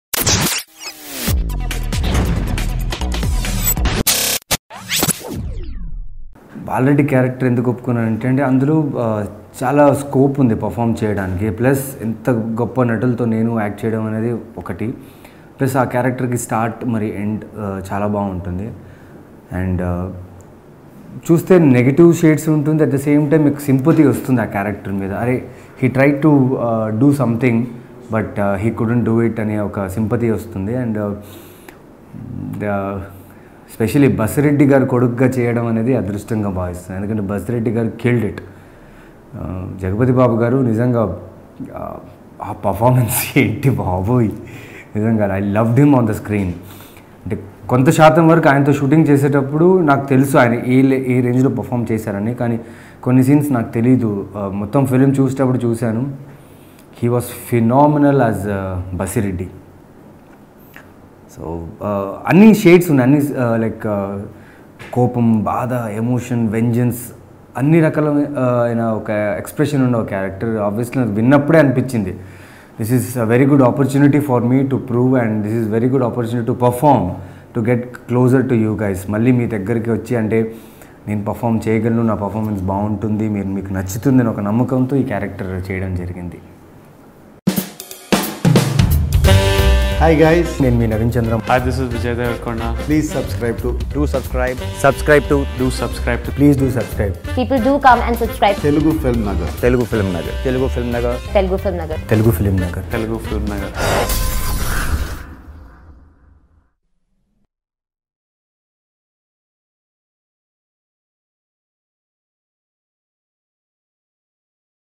Naveen Chandra about Jagapathi Babu | Aravindha Sametha Interview | Jr NTR | Sunil |Telugu FilmNagar
Naveen Chandra about Jagapathi Babu, Aravindha Sametha Interview on Telugu FilmNagar. Aravindha Sametha 2018 Telugu Movie ft. Jr NTR, Pooja Hegde, Sunil, and Jagapathi Babu. Directed by Trivikram and Produced by S Radha Krishna. Music Composed by Thaman S. AravindhaSametha NaveenChandra JrNTR Trivikram PoojaHegde Jagapathi Babu